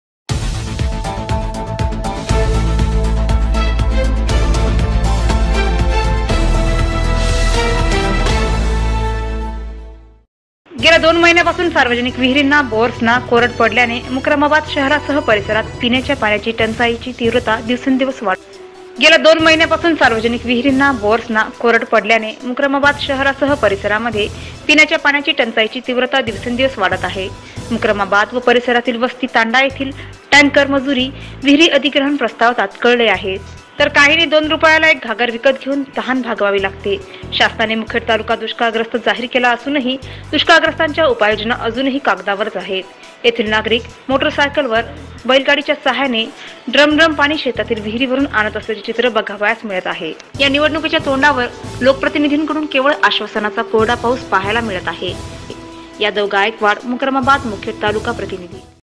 तापत्या उन्हात तासतांस थांबल्यावार भागते लोकांची तहान...
दोन रुपयाला एक घागर ...उन्हात पाणी भरत आसलेल्या अनेक महीला आजारी परिसरातील परिस्थीती
           यादव गायकवाड मुक्रमाबाद मुखेड तालुका प्रतिनिधी
                गेल्या दोन महिण्यापासुन सार्वजनिक विहिरींना, बोअर्संना कोरड पडल्याने मुक्रमाबाद शहरासह परीसरात पिण्याच्या पाण्याच्या टंचाईची तीव्रता दिवसेंदिवस वाढत आहे .
           मुक्रमाबाद  व परिसरातील वस्ती तांडा येथील टँकर मजुरी , विहीर अधिग्रहण प्रस्ताव ताटकळले आहेत .यामुळे पिण्याच्या पाण्यासाठी नागरिकांना दिवसरात्र भटकंती करण्याची वेळ आली आहे .
 मुक्रमाबाद  तसेच  आसपास चे वस्ती तांडा परिसरातील ग्रामस्थांना शेतातील विहिरींचा आधार घ्यावा लागत आहे. तर काहींनी दोन रुपयाला एक घागर विकत घेवुन तहान भागवावी लागते.शासनाने मुखेड तालुका दुष्काळ ग्रस्त जाहीर केला असूनही दुष्काळ ग्रस्ताच्या उपाय योजना अजुनही कागदावरच आहेत . येथील नागरिक मोटार सायकलवर ,  बैलगाडीच्या सहाय्याने ड्रम-ड्रम पाणी शेतातील विहिरीवरून आणत असल्याचे चित्र बघावयास मिळत आहे. मार्च मध्ये ही परिस्तिथी तर  एप्रिल,मे  महिन्यात येथे जलसंकट रौद्ररुप धारण करण्याची शक्यता वर्तविली जात असल्याने जि .प .पंचायत समिती ,तालुक्यातील लोकप्रतिनिधी शासन , प्रशासनाने तात्काळ पाणीटंचाई संदर्भात योग्य पाऊल उचलण्याची गरज आहे.
मुक्रमाबाद  तसेच  आसपास चे वस्ती तांडा परिसरातील नागरिकांना सध्या जल समस्येचा सामना करावा लागत आहे.अद्यापही जि .प च्या पाणीपुरवठा विभागाने पिण्याच्या पाण्यासाठी टँकर ची सुविधा उपलब्ध करून दिलेली नाही.पाणीटंचाईची तीव्र समस्या तालुक्यात असतानाही लोकप्रतिनिधी विधानसभा  आणि लोकसभा निवडणुकीची मोर्चे बांधणीत व्यस्त दिसून येत आहे .या निवडणुकीच्या तोंडावर लोकप्रतिनिधीकडून केवळ आश्वासनाचा कोरडा पाऊस पहायला मिळत आहे .मुक्रमाबाद  तसेच  आसपास चे वस्ती तांडा परिसरातील नागरिक सांगतात कि पुर्वी  १००ते १५० फुट खोलीवर खोदकामादरम्यान येथे पाणी लागत असे. पण आता मात्र भुगर्भातील जलपातळी  खूपच खाली  गेल्याचे ५०० ते ७०० फूट बोअर पाडूनही  येथे पाण्याचा काही पत्ता नाही.यामुळे गावातील बहूतांश विहिरी तळ दाखवत असून काही विहिरींना कोरड पडली आहे. भविष्यातील जलसंकट लक्षात घेऊन या भागात शासकीय नियमांना फाटा देत बोअर केली जात आहे.
इतकेच नव्हे तर काही बोअर करणाऱ्यांकडून सध्या नागरिकांच्या अडचणीचा फायदाच घेतल्या जात असल्याचे दिसून येते. विहिरीला कोरड पडल्याने काहींना शेतातील विहिरींचा आधार घ्यावा लागत आहे. ज्यांची बोअर करण्याची आर्थिक स्थिती नाही अशांना तीन ते चार किमी अंतरावरून बैलगाडीवर मोठाले ड्रम लादून त्याद्वारे शेतातील विहिरींवरून पाणी आणावे लागत आहे. . ऐन मार्च महिन्यात  ही परिस्थिती असून  एप्रिल मे मध्ये ही परिस्थिती विदारक रुप धारण करण्याची शक्यता वर्तविली जात आहे.मुक्रमाबाद  तसेच  आसपास चे वस्ती तांडा परिसरातील  सार्वजनिक विहिरीला कोरड पडल्याने विहीरीतील पाणी काढण्यासाठी जिव मुठीत धरून पाणी काढावे लागत आहे .गावातील विहीर तळ गाटल्याने शेतातील विहिरीचा आधार घ्यावा लागत आहे. बैलगाडीवर, मोटारसायकलवर पाणी आणावे लागत आहे.